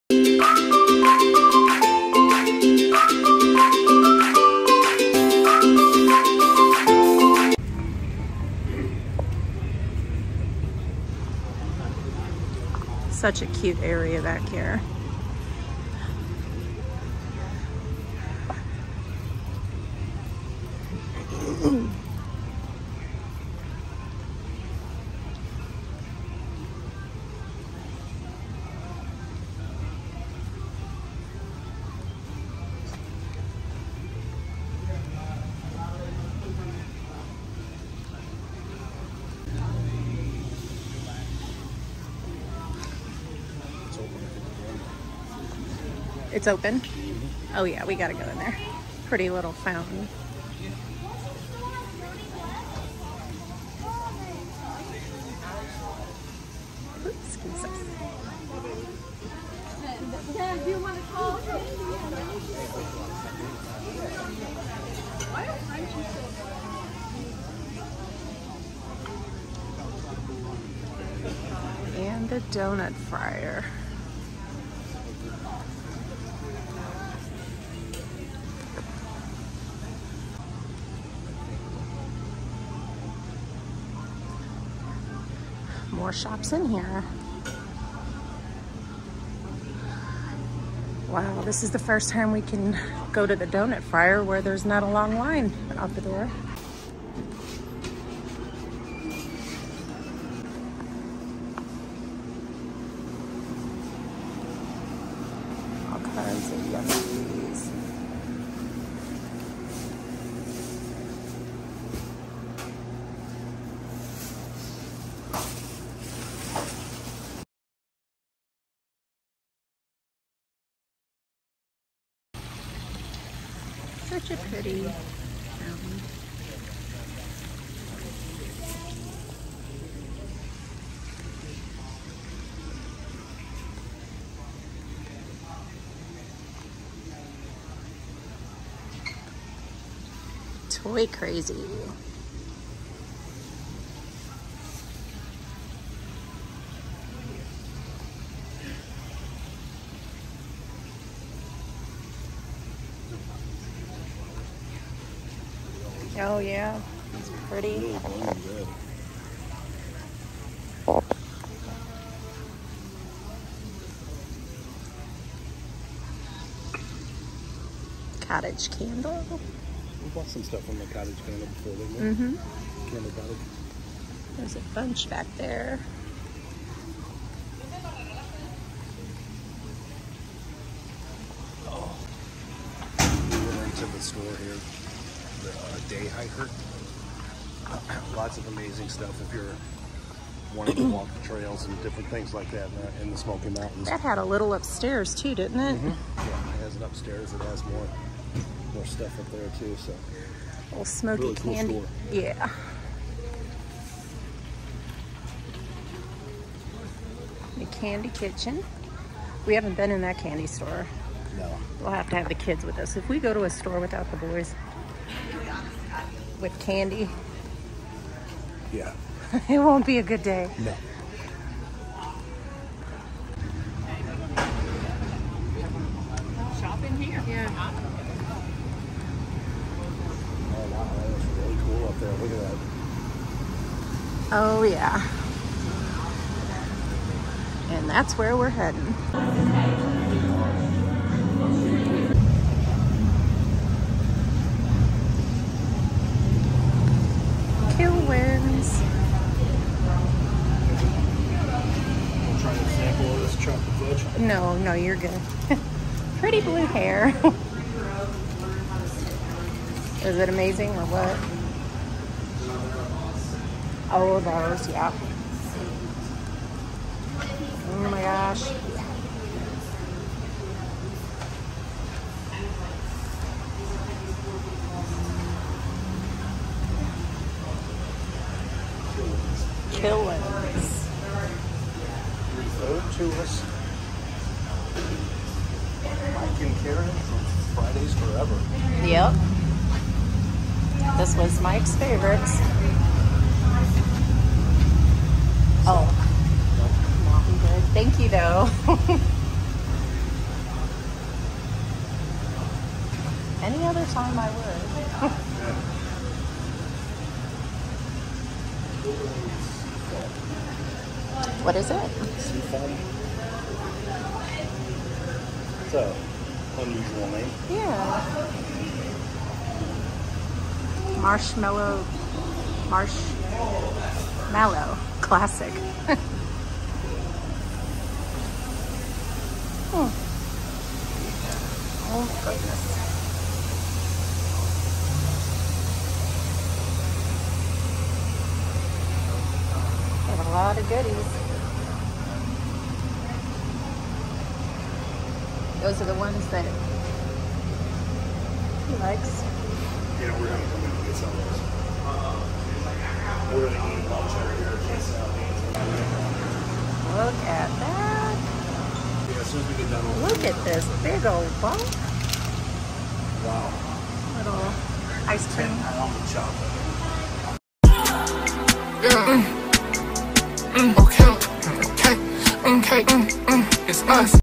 Exploring Gatlinburg's Sweetest Delights: Kilwins & The Donut Friar
Kilwins delicious chocolate and a quick treat at the Donut Friar!

Copy & paste in browser if you can’t click link :)